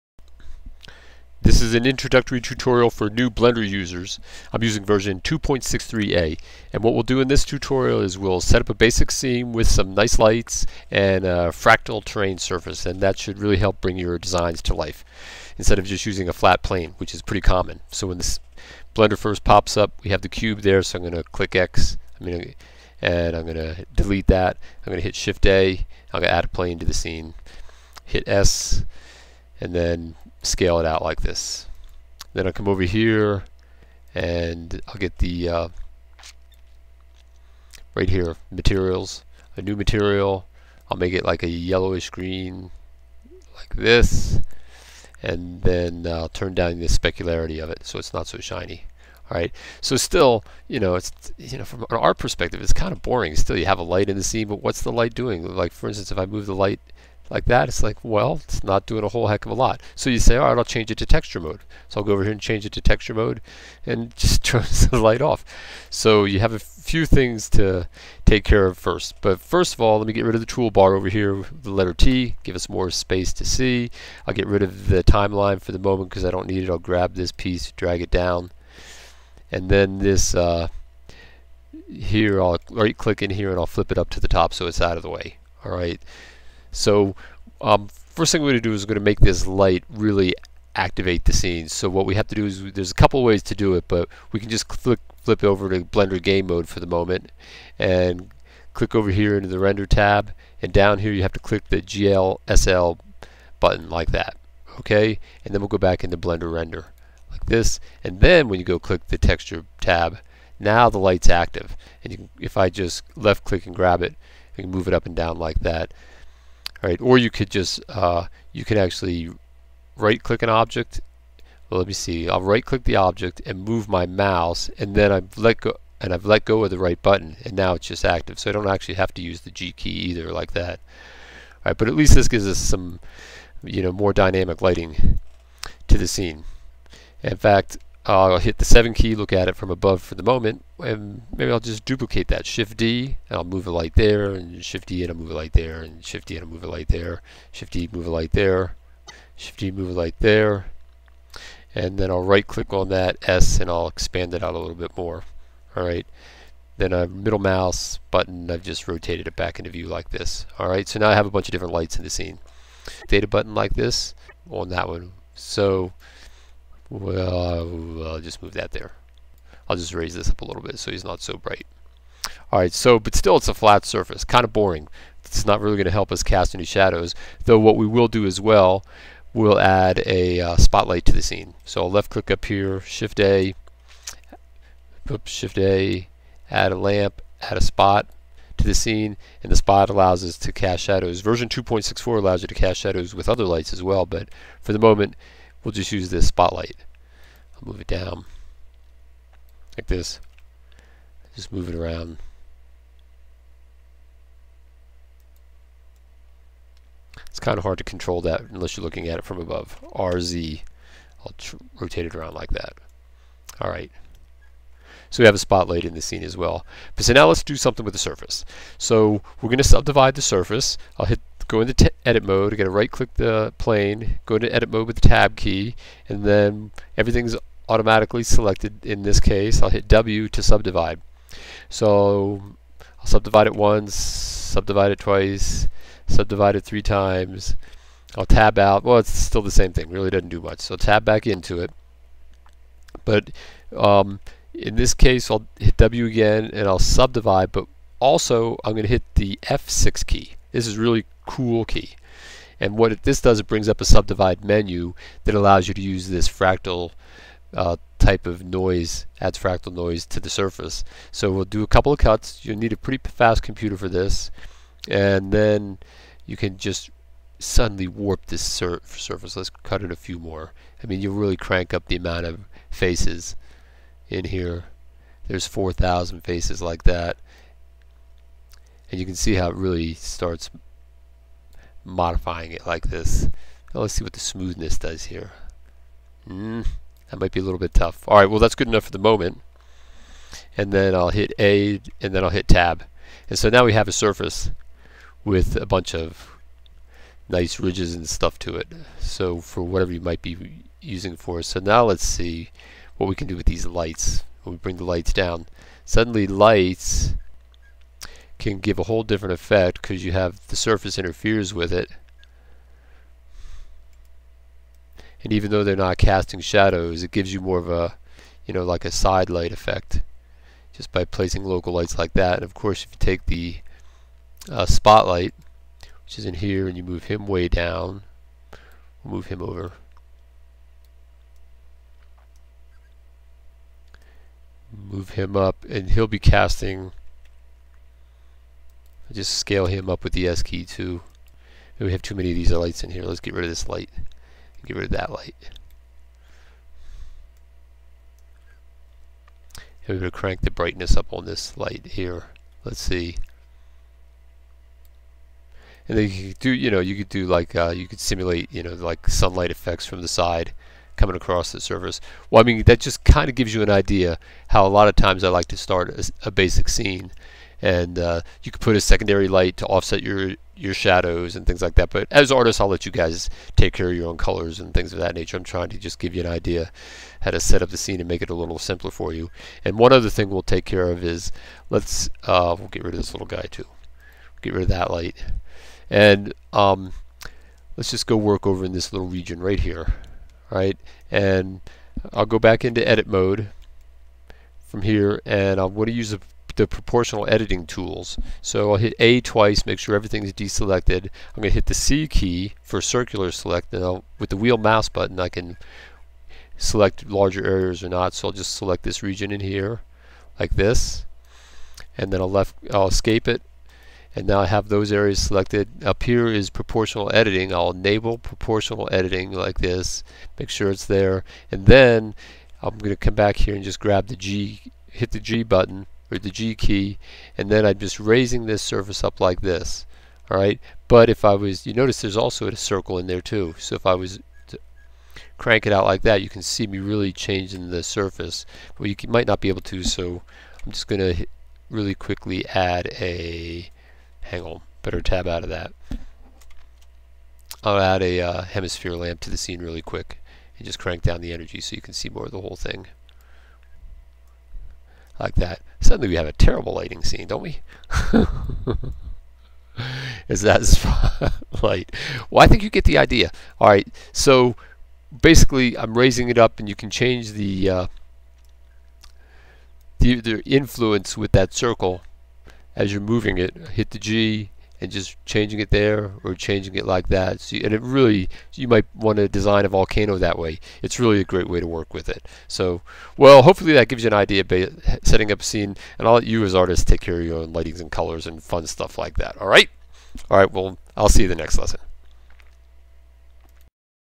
Blender 2.63a Tutorial - Setting up a Fractal Terrain Surface
Introductory tutorial for new Blender users.